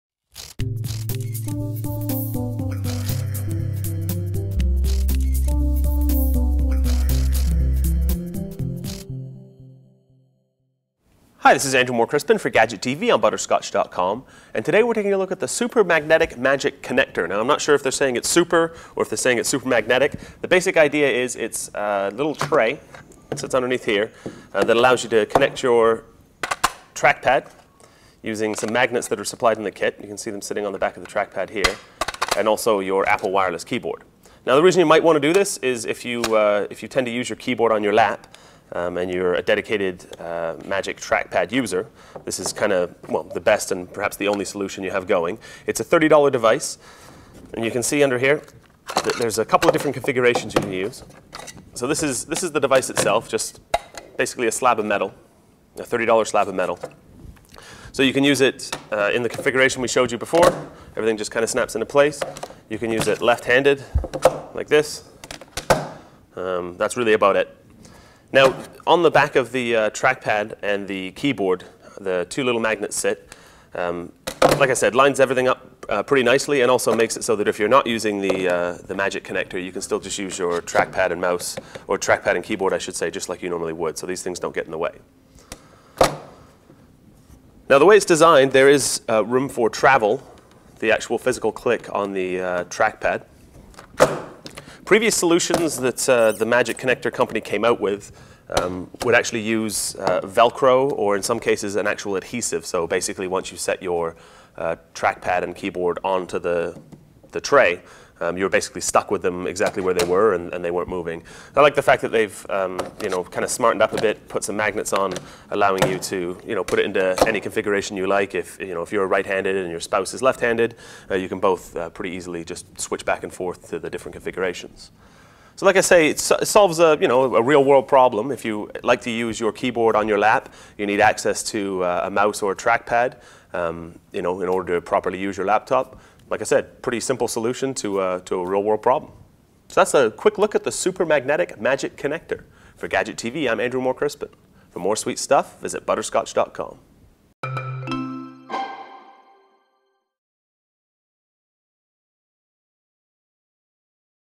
Super Magnetic Magic Connector video review
Rather than trying to balance the small Apple Wireless Keyboard on one knee and the Magic Trackpad on the other, the Magic Connector ties them both together. Neodynium magnets on the Connector and on your peripherals lock them in place @ butterscotch.com